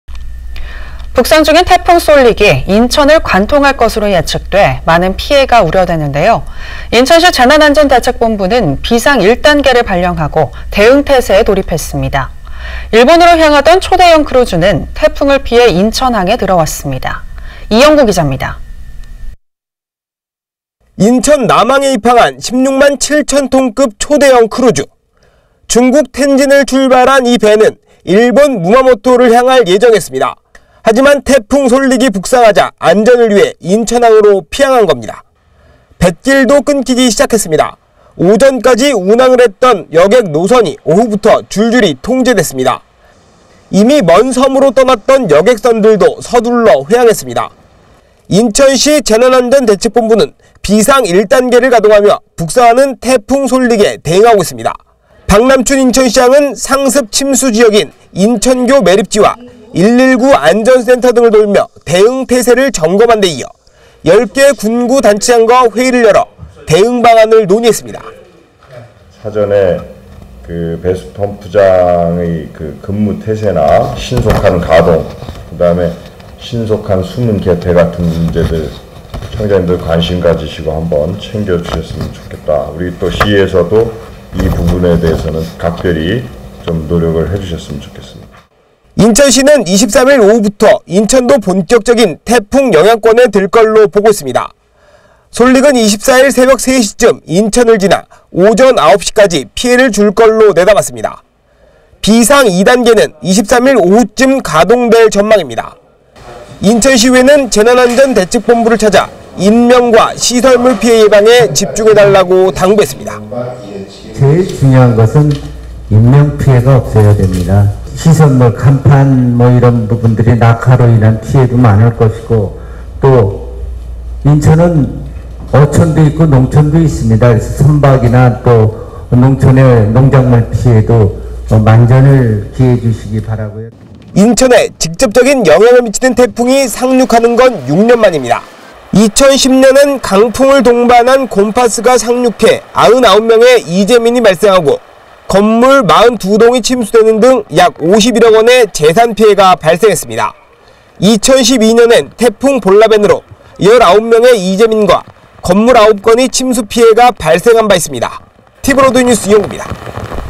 태풍 솔릭 상륙 임박…인천 '비상 1단계' 발령 - 티브로드 인천방송
정유현 앵커)
북상 중인 태풍 '솔릭'은 인천을 관통할 것으로 예측돼 많은 피해가 우려되는데요.
인천시 재난안전대책본부는 ‘비상 1단계’를 발령하고 대응 태세에 돌입했습니다.
일본으로 향하던 초대형 크루즈는 태풍을 피해 인천항에 들어왔습니다.

리포트)
인천 남항에 입항한 16만7천 톤급 초대형 크루즈.

중국 텐진을 출발한 '오베이션 오브 더 시즈'는 일본 무마모토를 향할 예정이었습니다.

하지만 태풍 솔릭이 북상하자 안전을 위해 인천항으로 피항한 겁니다.

뱃길도 끊기기 시작했습니다.

오전까지 운항을 했던 여객 노선이 오후부터 줄줄이 통제됐습니다.

이미 먼 섬으로 떠났던 여객선들도 서둘러 회항했습니다.

인천시 재난안전대책본부는 비상 1단계를 가동하며 북상하는 태풍 솔릭에 대응하고 있습니다.

박남춘 시장은 상습침수 지역인 인천교매립지와 119안전센터 등을 돌며 대응태세를 점검한 데 이어, 10개 군·구 단체장과 회의를 열어 대응 방안을 논의했습니다.

SYNC.1) 박남춘 / 인천시장
"사전에 배수펌프장의 근무태세나 신속한 가동, 신속한 수문 개폐 등의 문제를 청장님들이 관심을 두고 챙겨주셨으면 좋겠다, 인천시에서도 이 부분에 대해서는 각별히 노력을 해주셨으면 좋겠습니다."

인천시는 내일(23일) 오후부터 인천도 본격적인 태풍 영향권에 들 걸로 보고 있습니다.

솔릭은 24일 새벽 3시쯤 인천을 지나 오전 9시까지 피해를 줄 걸로 내다봤습니다.

'비상 2단계'는 내일(23일) 오후쯤 가동될 전망입니다.

인천시의회는 재난안전대책본부를 찾아 인명과 시설물 피해 예방에 집중해 달라고 당부했습니다.

SYNC..2) 안병배 / 인천시의회 부의장
"제일 중요한 것은 인명 피해가 없어야 합니다. 시설물 간판 등이 낙하로 인한 피해가 많을 것이고, 또 인천은 어촌도 농촌도 있습니다. 선박이나 농작물 피해 예방에도 만전을 기해 주시길 바라고요"

인천에 직접적인 영향을 미치는 태풍이 상륙하는 건 6년 만입니다.

2010년엔 강풍을 동반한 곤파스가 상륙해 99명의 이재민이 발생하고 건물 42동이 침수되는 등 약 51억 원의 재산 피해가 발생했습니다.

2012년엔 태풍 볼라벤으로 19명의 이재민과 건물 6건의 침수 피해가 발생한 바 있습니다.

티브로드 뉴스 이형구입니다.

영상취재/편집 : 이태주